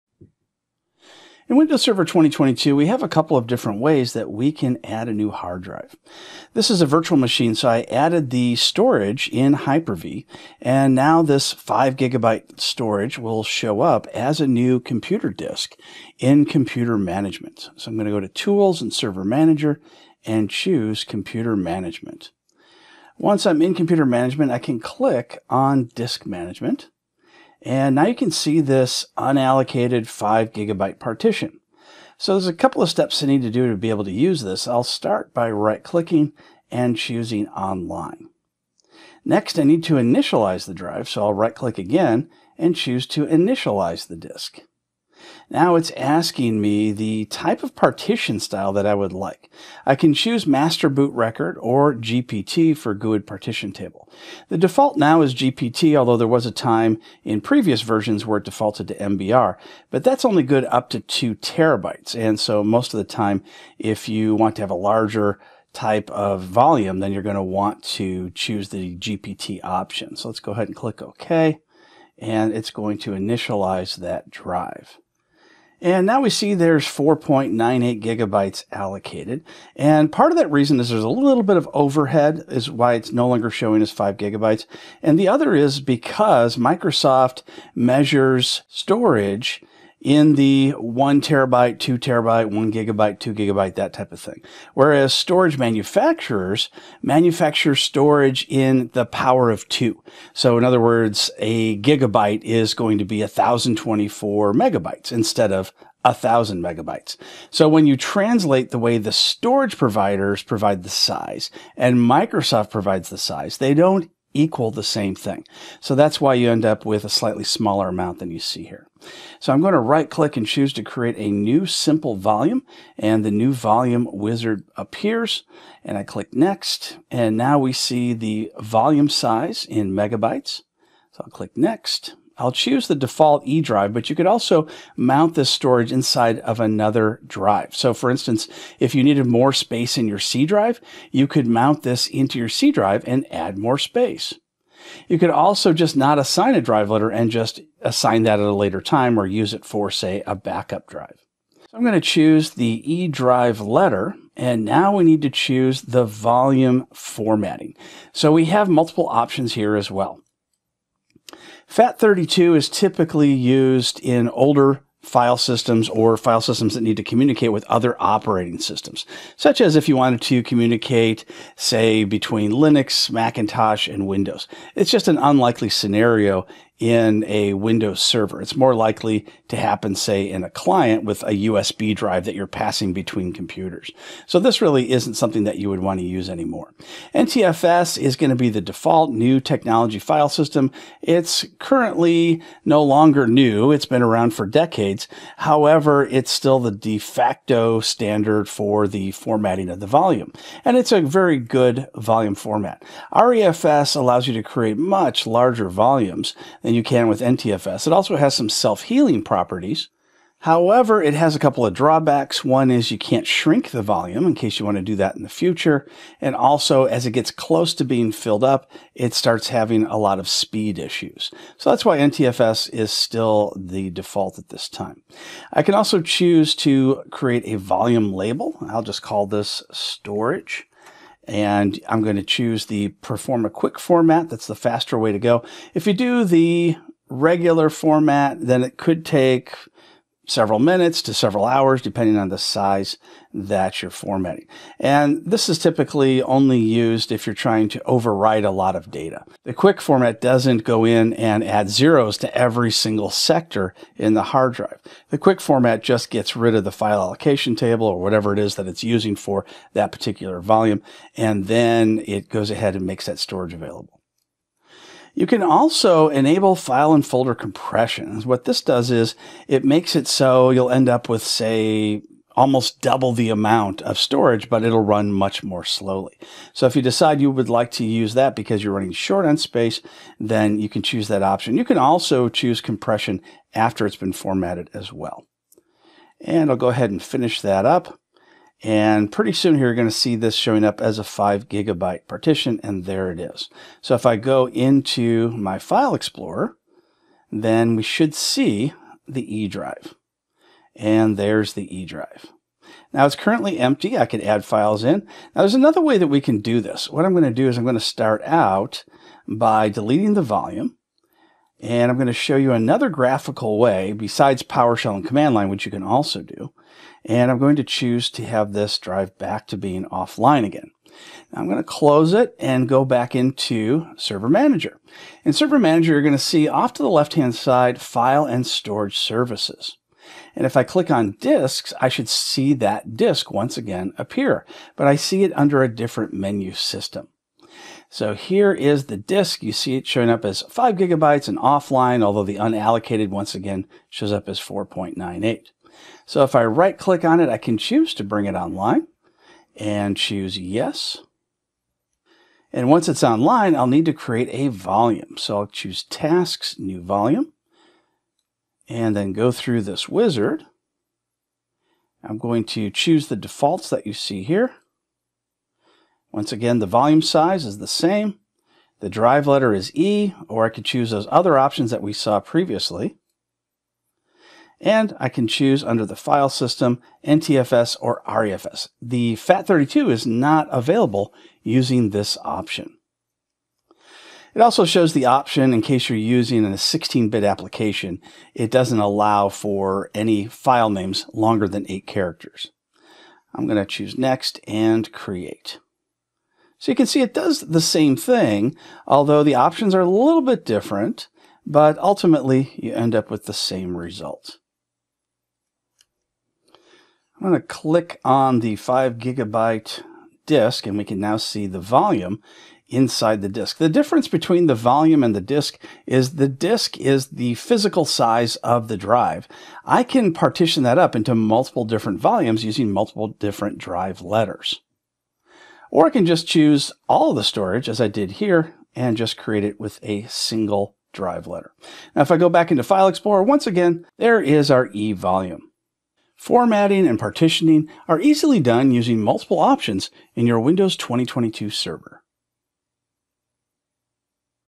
Learn How to Create a Disk Volume Using Windows Server 2022
Professor Robert McMillen shows you how to Create a Disk Volume in Windows Server 2022.